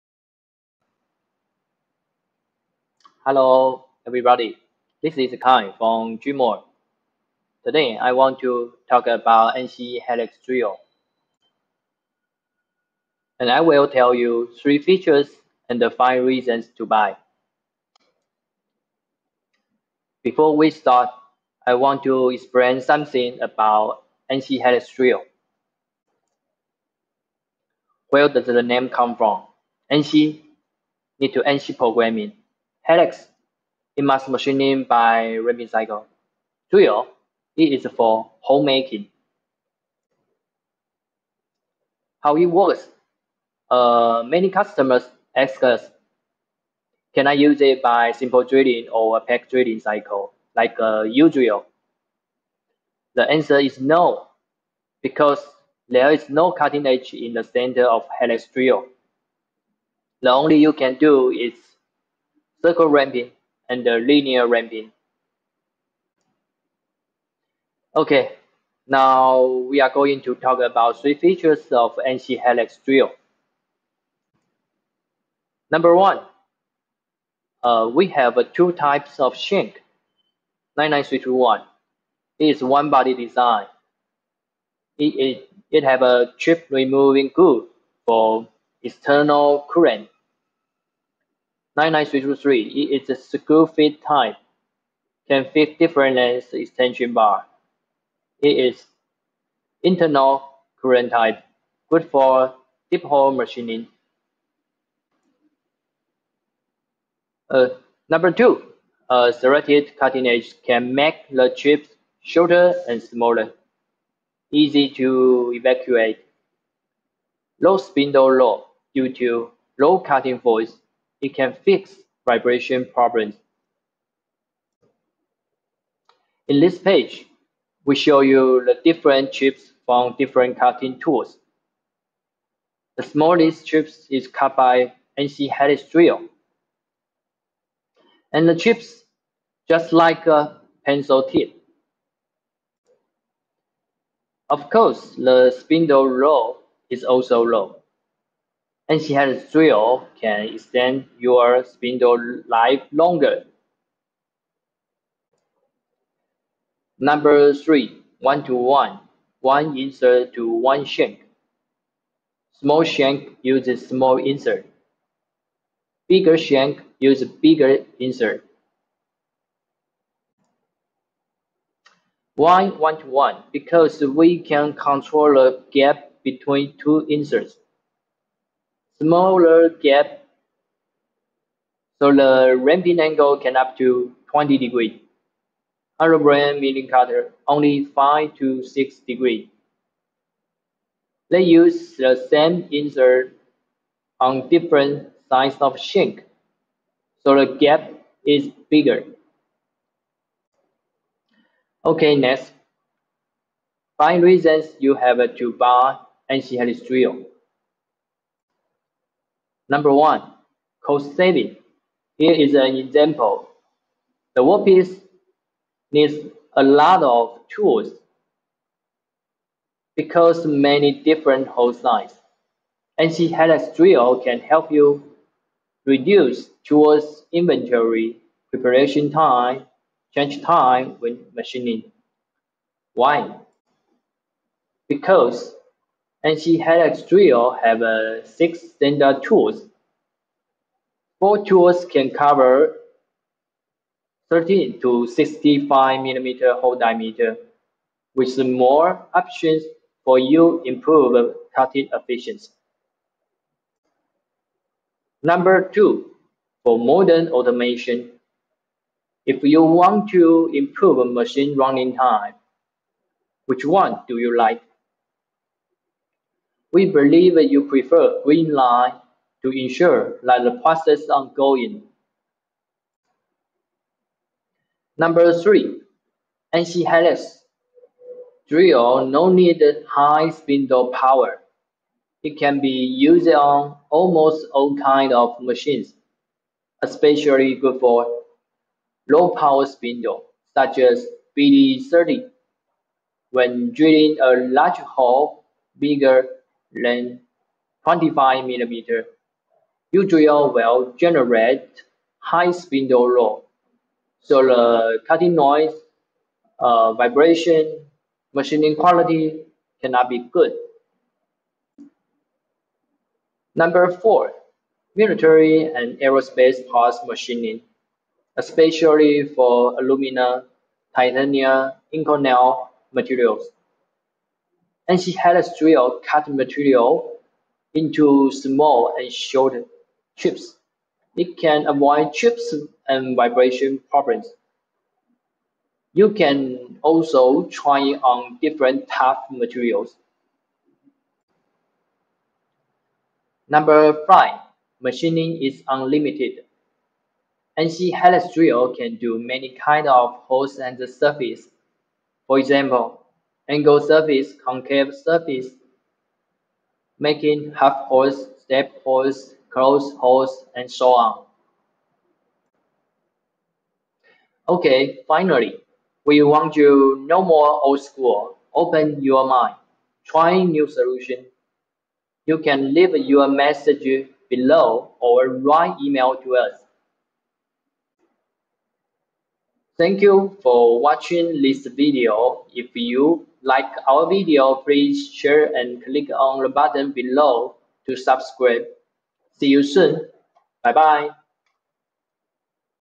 presentation of Nine9 NC Helix Drill- a problem solver of hole-making
● Nine9 NC Helix Drill cuts material by helical interpolation;
One tool performs multiple functions. the serrated cutting edge makes cutting chip short and easily to be removed.
● Lower Spindle Power Consumption. Thanks to the small cutting load of the serrated cutting edge and helical interpolation, low power consumption of the spindle is required. Circular ramping milling, maximum ramping angle is 20°.
● Special geometry insert to cut dierent materials. For almost all kind of materials, excellant for soft and long cutting chip materials!, such as low carbon steel, stainless steel, Titanium.
● Job has various hole diameters & Step Holes. Only six tools for drilling Ø13~Ø65 mm. The hole is cut by helical interpolation. Just one tool can machine dierent diameters and depth of holes.